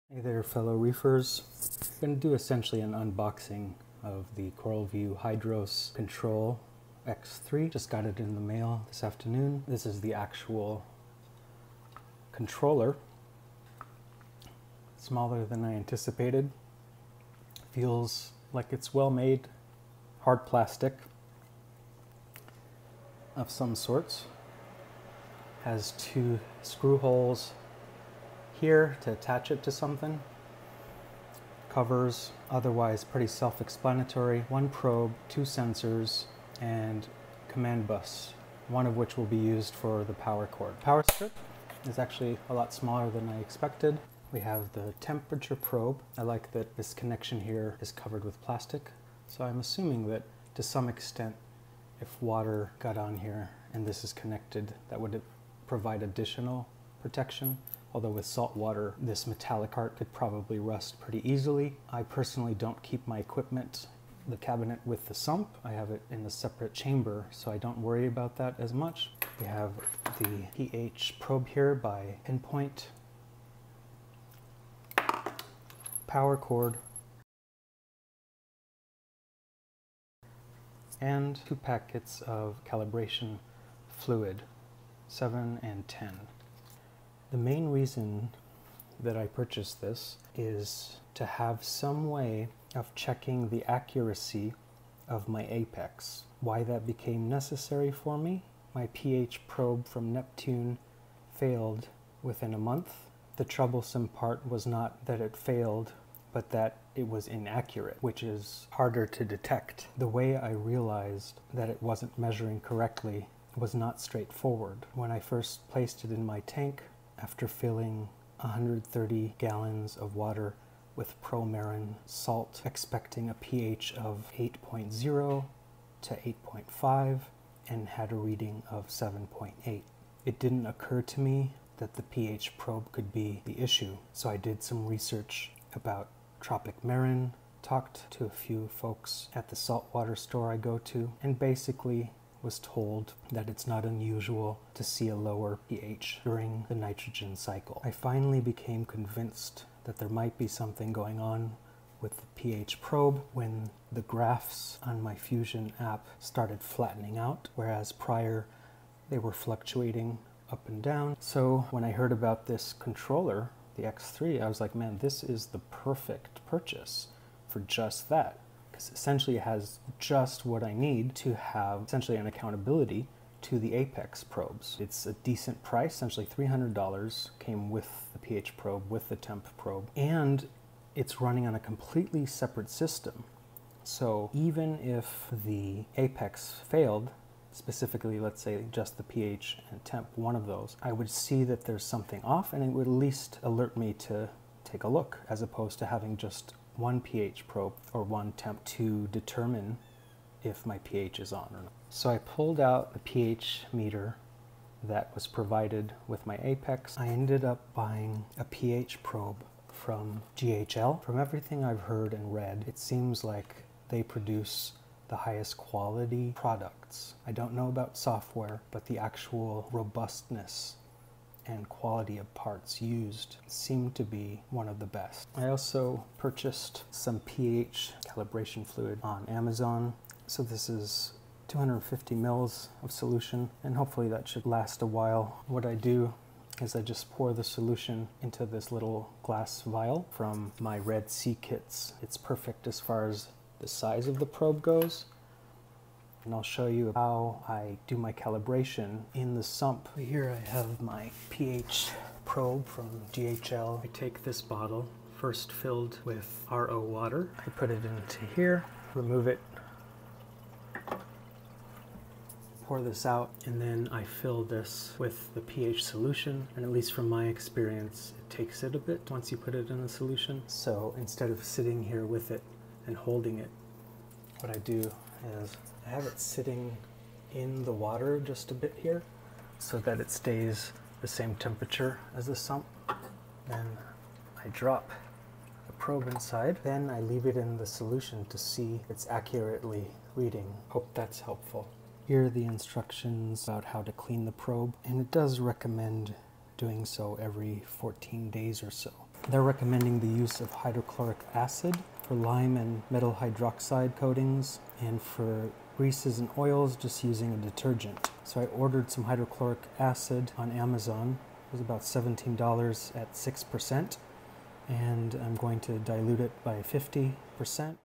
CoralVue HYDROS Control X3 Starter Pack and Why I Purchased it
Unboxing/ Apex pH probe failure/ How I am using the Hydros to prevent future probe failure.